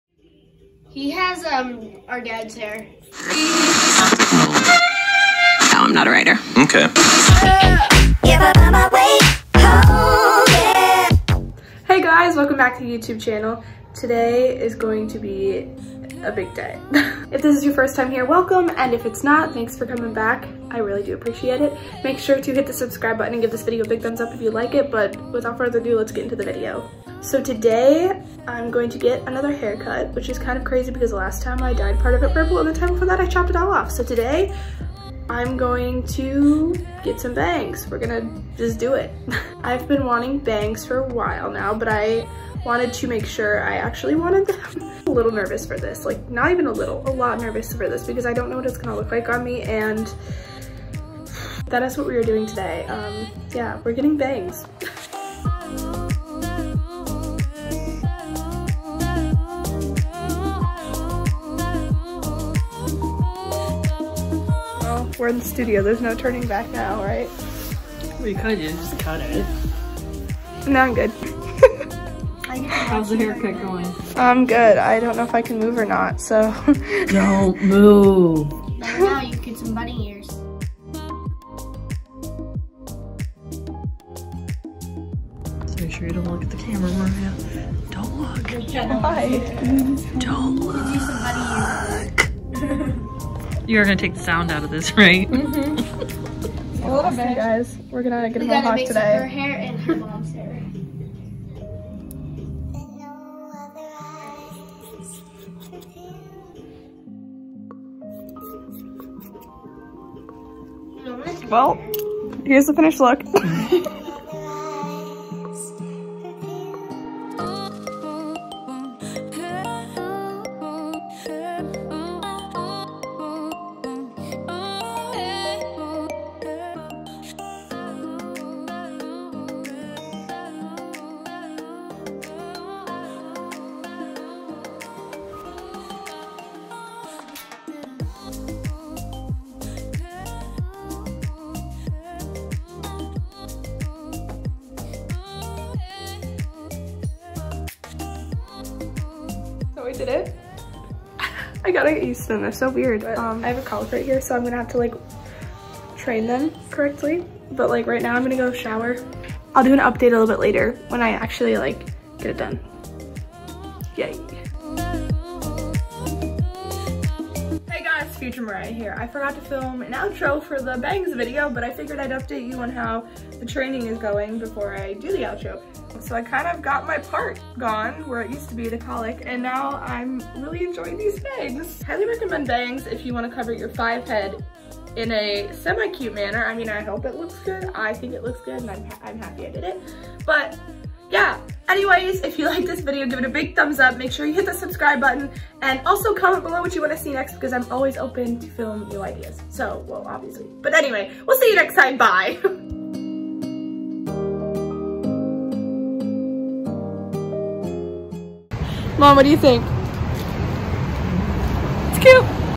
Joined the Bang Gang || VLOG 10
Hi all! I am back to Youtube after a month or so. With all of this social distancing and being at home, I am trying to set an allotment of time dedicated to youtube. ANYWAYS. A little while back I got bangs and vlogged it! I love them so far, and I hope you do too. Thanks y'all. ♡

⬇Other People⬇
Stylist (Studio 117) -  Stacie Hackbarth